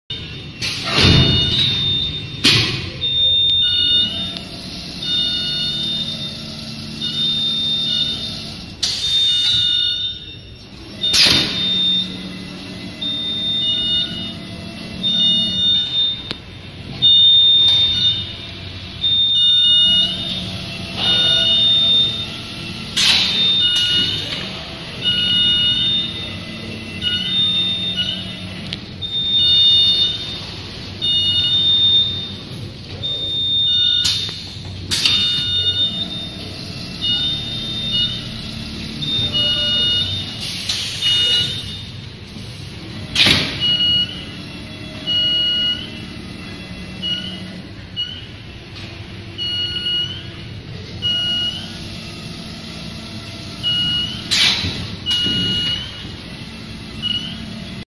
Automatic Stacking Robot
Customized fully stacking robot, XYZ 3- axis with servo motor driver, Can fulfill the Auto identifition, Auto picking, Auto pallet loading and Auto unloading the finished products.